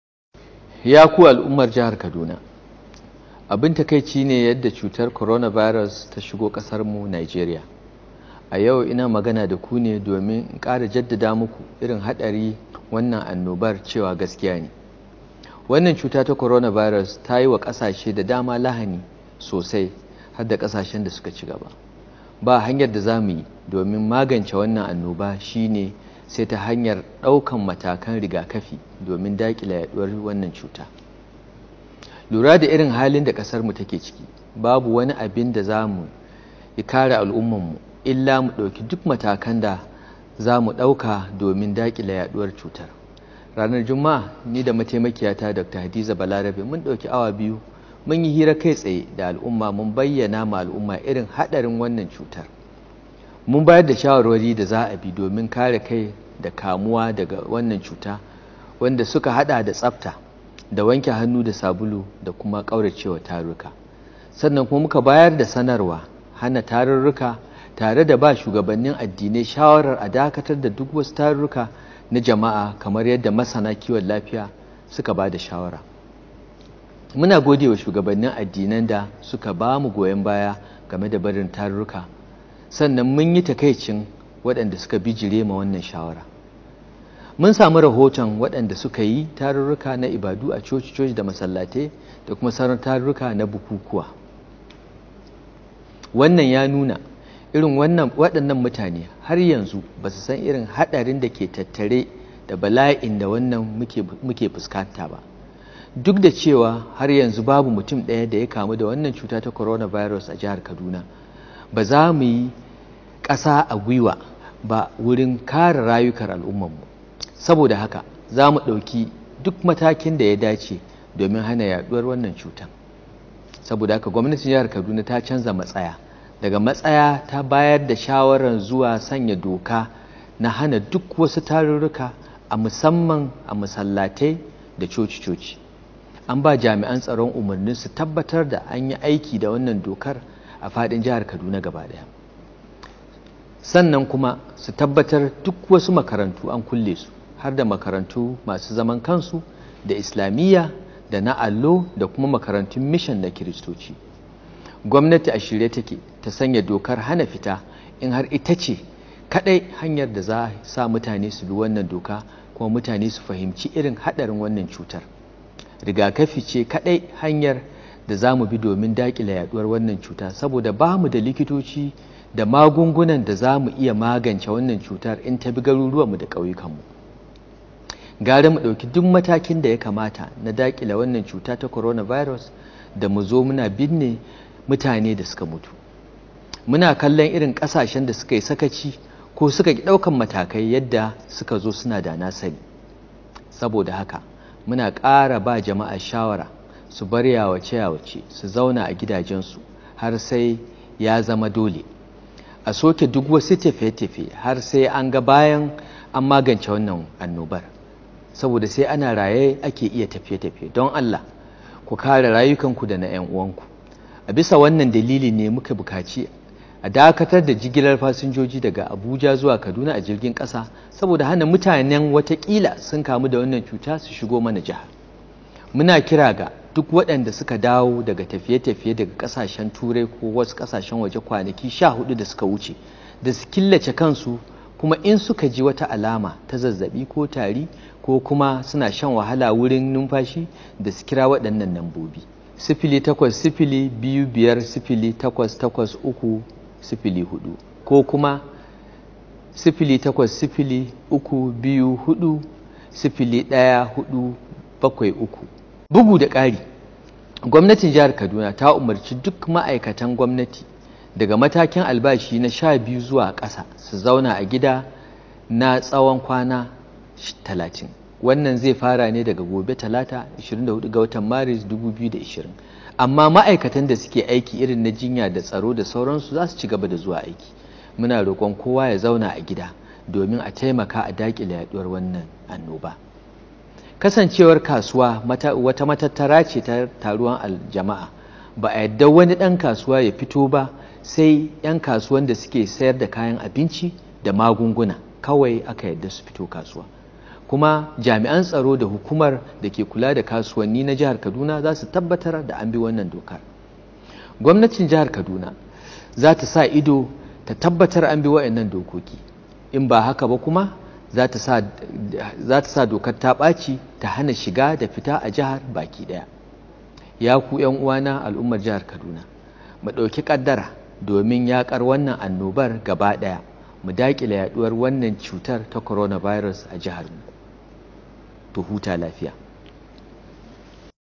Jawabin gwamnan Kaduna Mallam Nasiru El-Rufia Akan Corona Virus
Akan Corona Virus